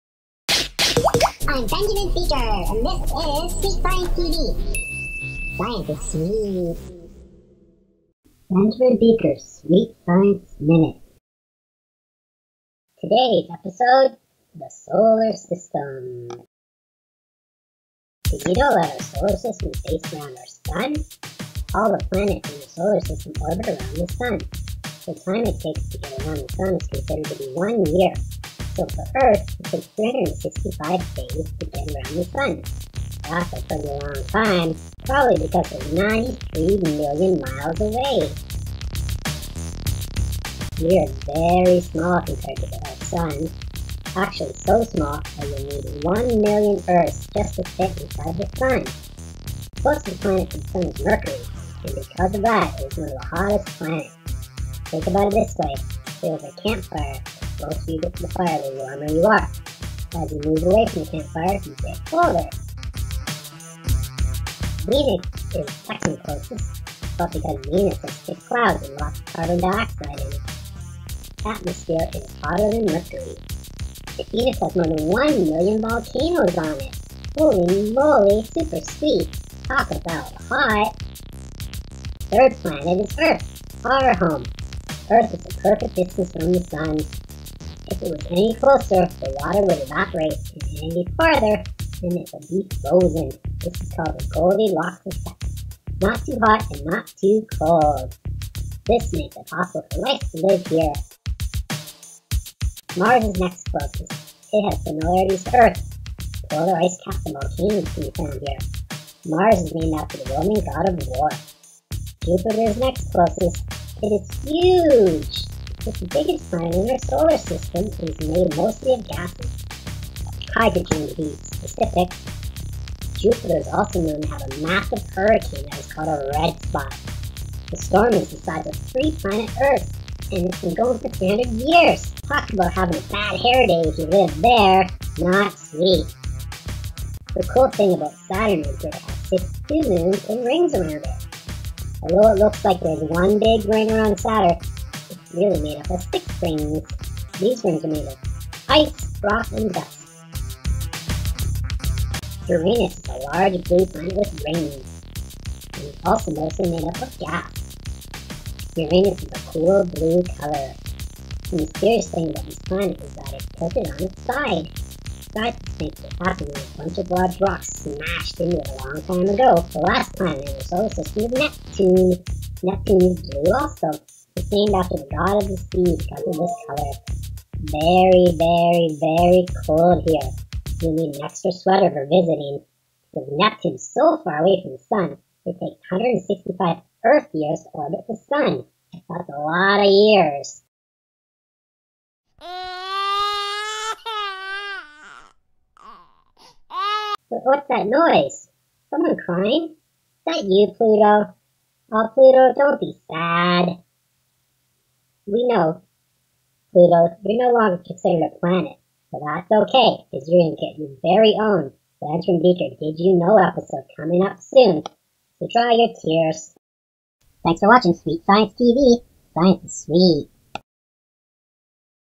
Benjamin Beaker's Sweet Science Minute - The Solar System
Come along with Benjamin Beaker as he takes you for a ride through our solar system.  Learn about all the planets, and find out why Pluto is so sad.